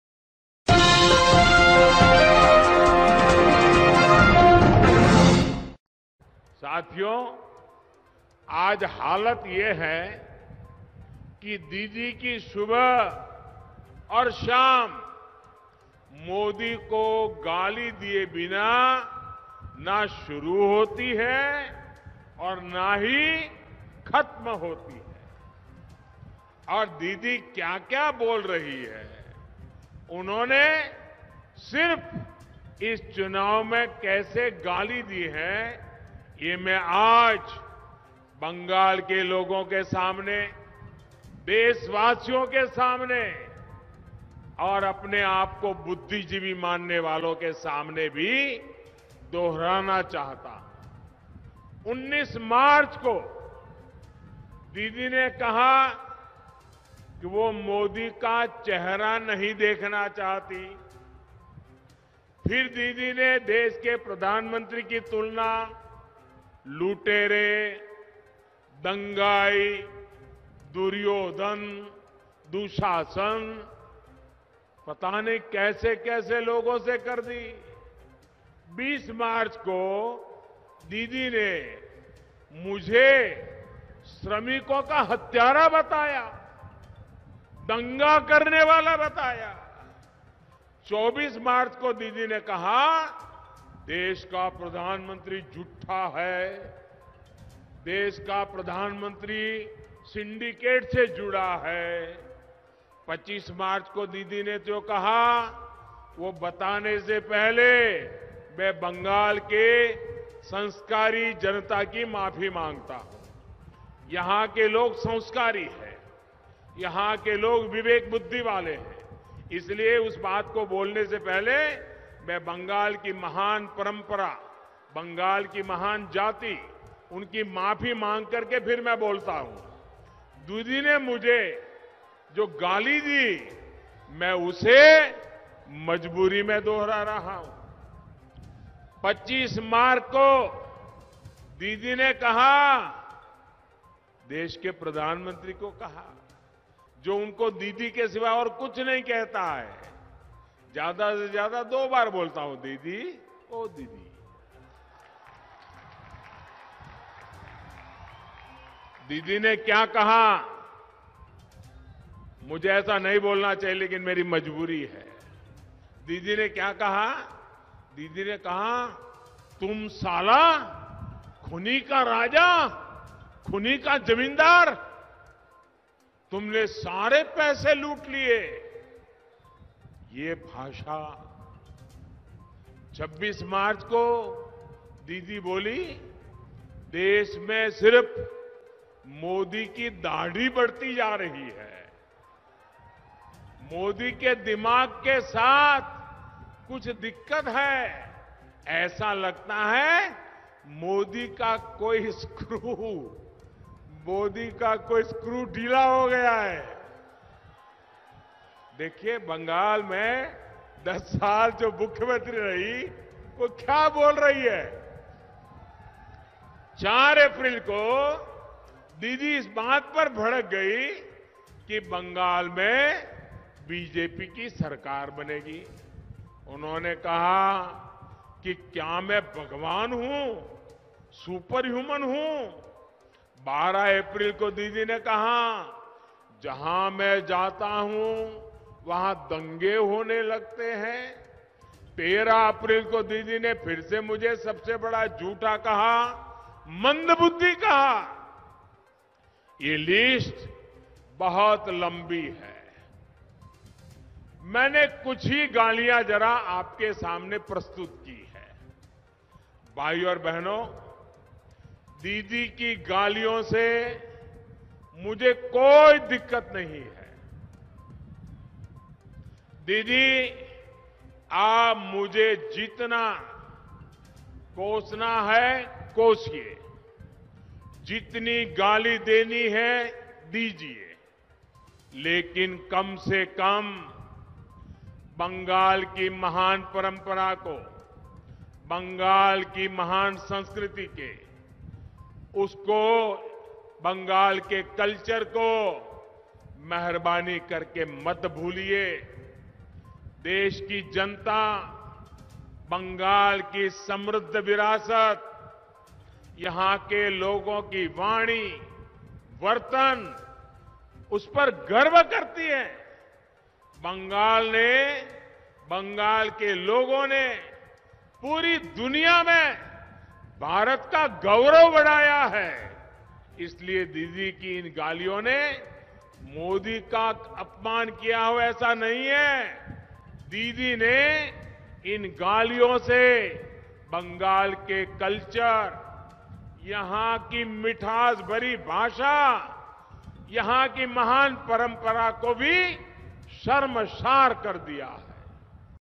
PM Narendra Modi Exposes Bengal CM Mamata Banerjee
Prime Minister Narendra Modi strongly attacked Chief Minister Mamata Banerjee for the election of West Bengal and said that Mamata Banerjee abuses a Prime Minister speaks wrong about a Prime Minister, today let me tell you what she says about me.